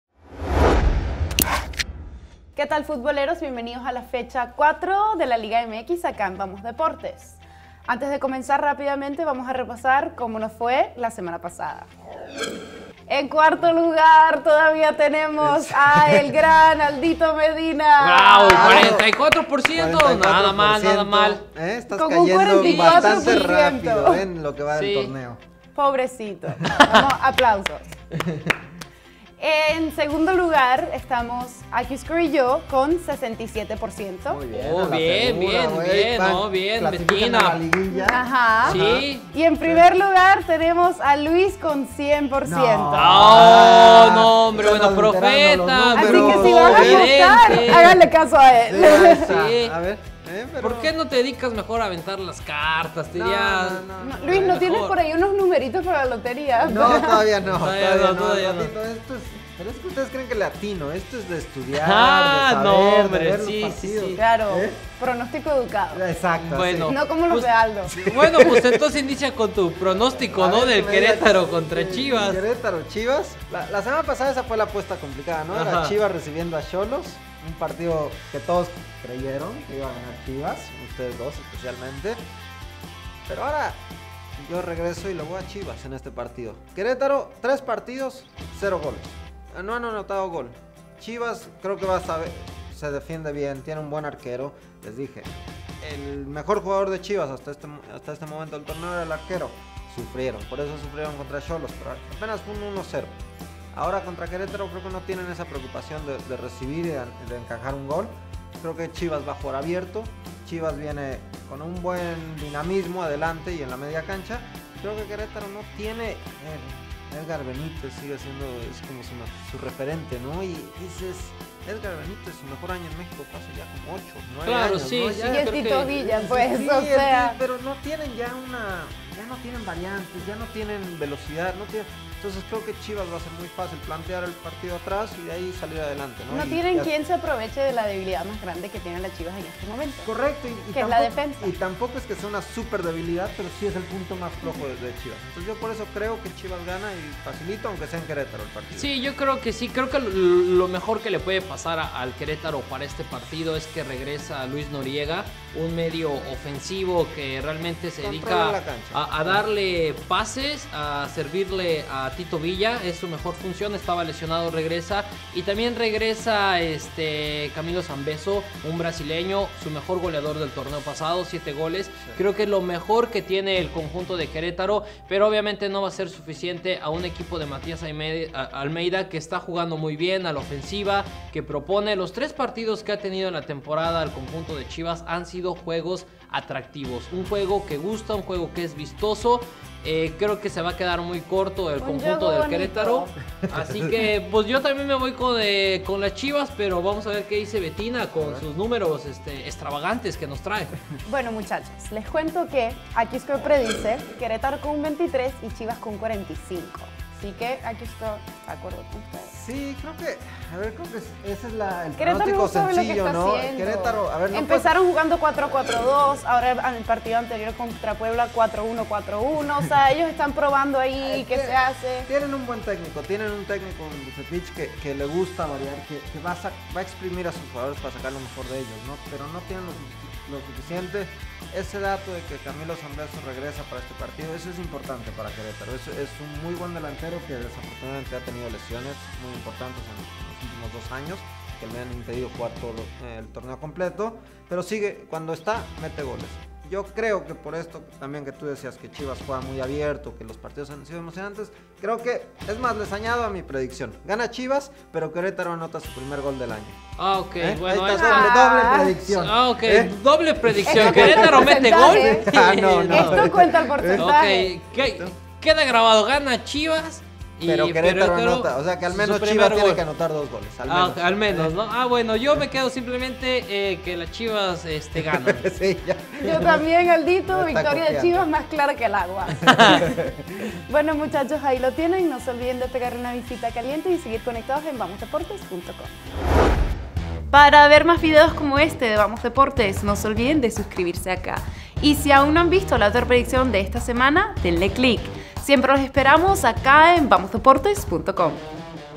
Querétaro vs Guadalajara (Clausura 2017 Jornada 4) - Predicción - VamosDeportes
Los Gallos Blancos reciben a las Chivas de Guadalajara en la Jornada 4 del Clausura 2017. No te pierdas nuestas predicciones, y las predicciones de Accuscore.